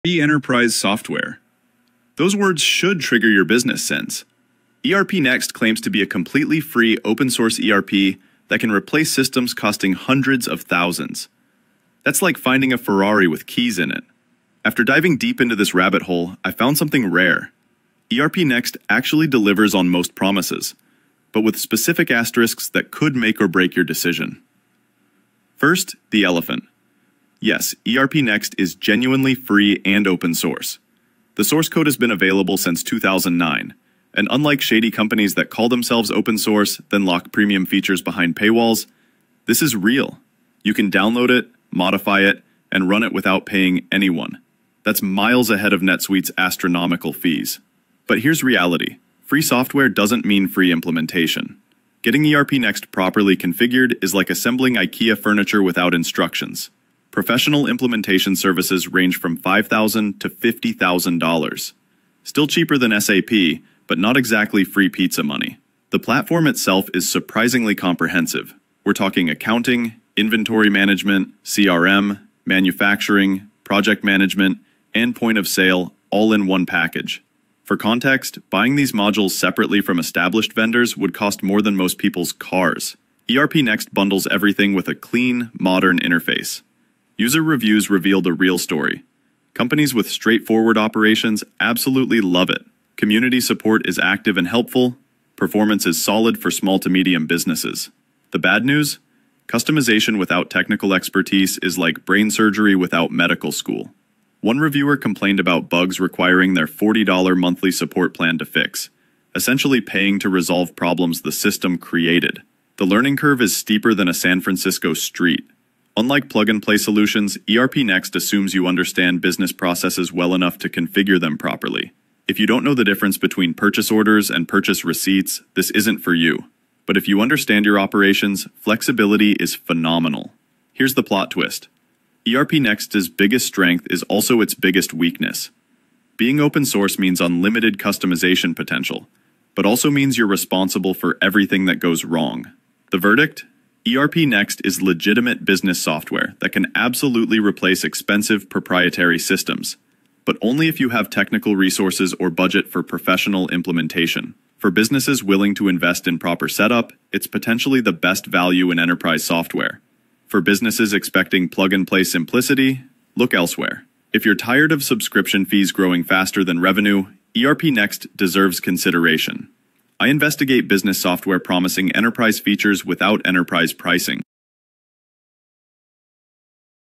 ERPNext Review: Free ERP That Replaces $100k Systems? (2026)
Is there really such a thing as completely free enterprise software that can compete with systems costing hundreds of thousands?

In this ERPNext review, I dive deep into what might be the most comprehensive open-source ERP solution available in 2026. You'll discover whether ERPNext can truly replace expensive systems like SAP and NetSuite, the hidden costs you need to know about, and who should (and shouldn't) consider this free alternative. I break down the real user experiences, implementation challenges, and whether the "free" promise actually holds up for businesses looking to escape escalating subscription fees.
From accounting and inventory management to CRM and manufacturing modules, ERPNext bundles enterprise features that would typically cost more than a luxury car when purchased separately. But as I reveal in this honest review, free software doesn't always mean free implementation – and the learning curve might surprise you.
🛠️ Tools Mentioned:

ERPNext - Open-source ERP platform
NetSuite - Cloud-based ERP system
SAP - Enterprise resource planning software